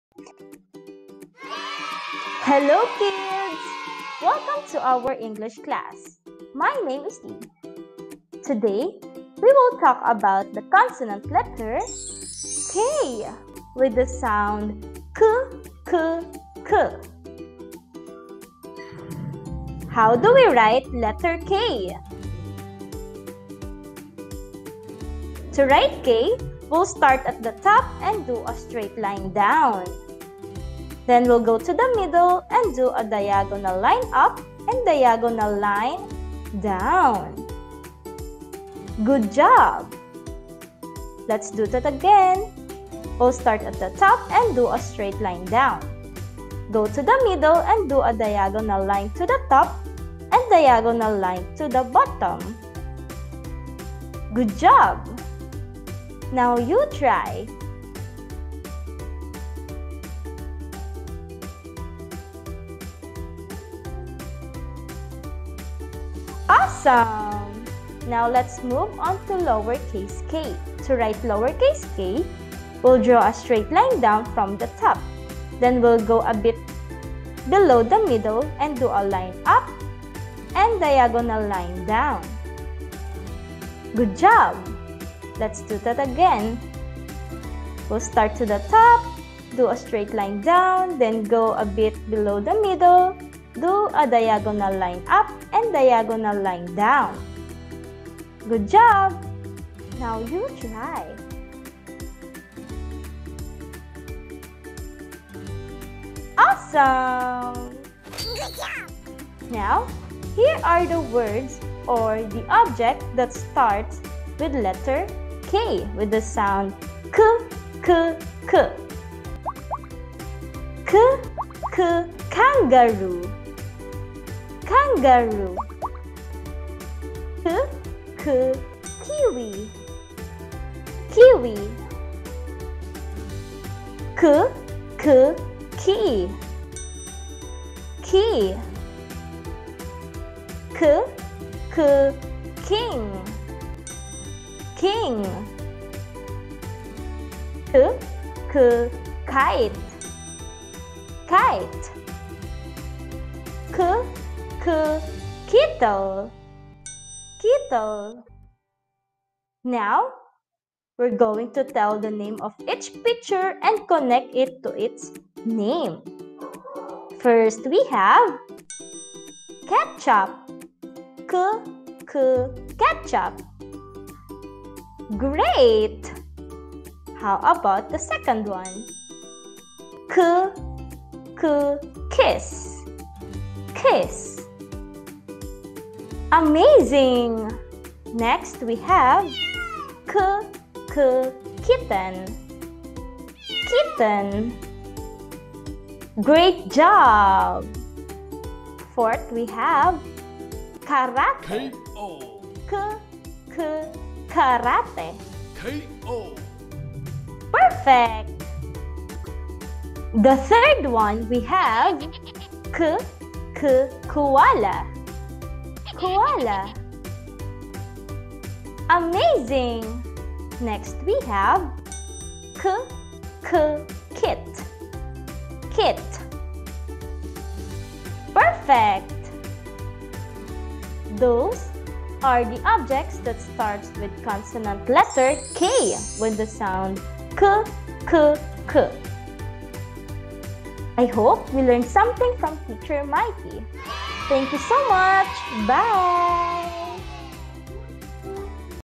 Consonant Letter Kk- Lesson Video
You will learn how to write uppercase and lowercase Kk, read words that starts with Kk and name objects that starts with Letter Kk.